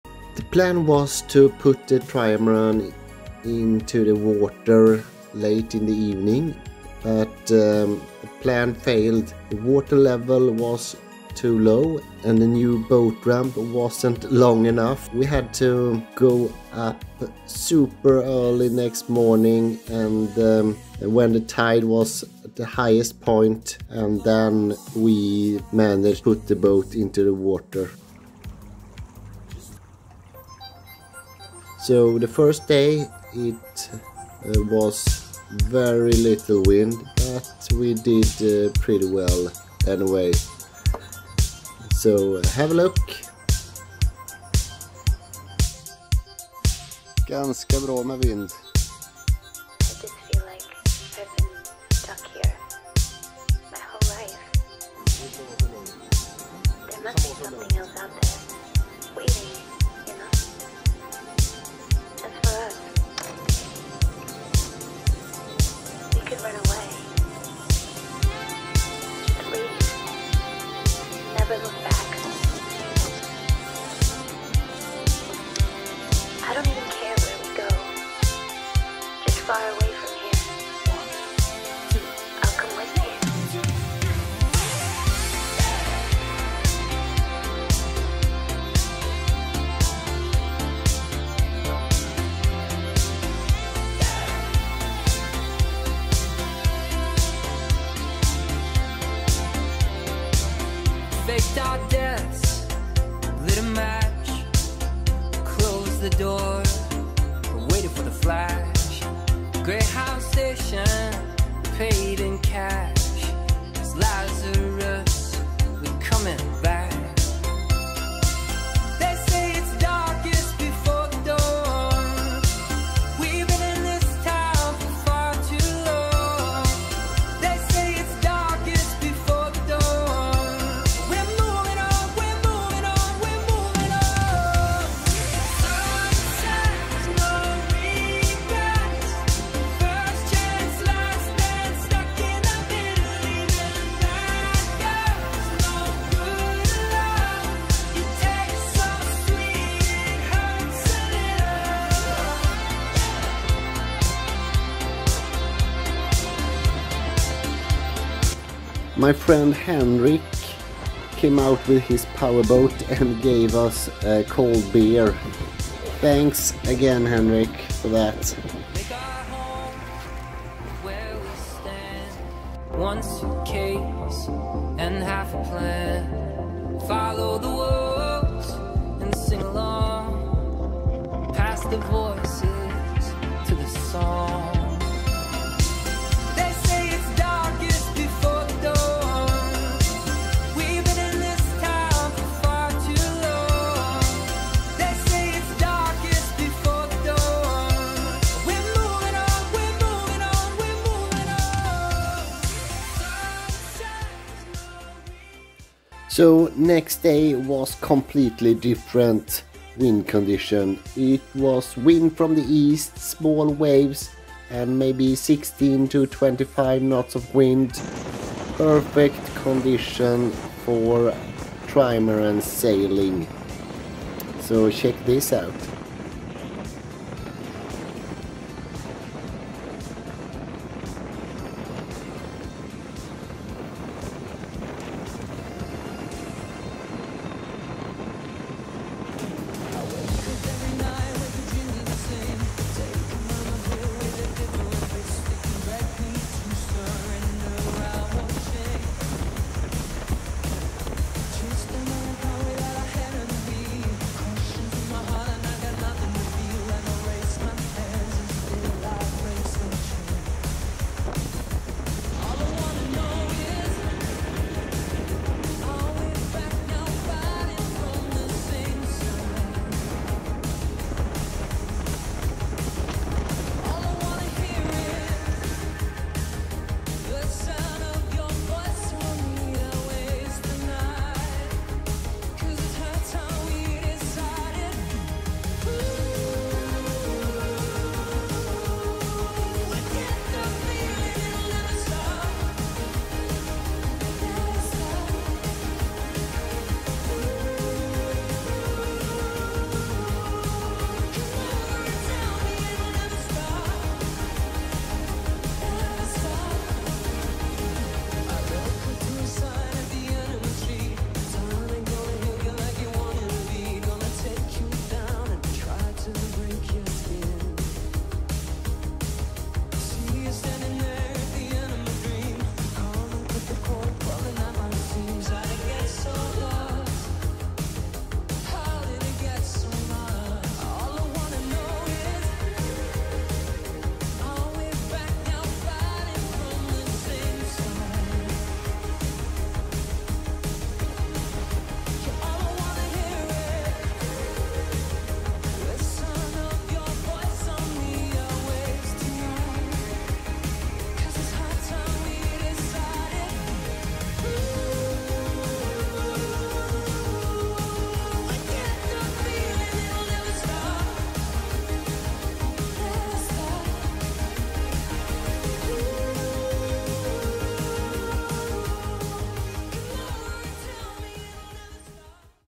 On stranger tides, trimaran sailing
First sailing of the season!!!
First days, no wind, second day,  A LOT!!!
Sailing my Dash 750 with my dad, on the Swedish west coast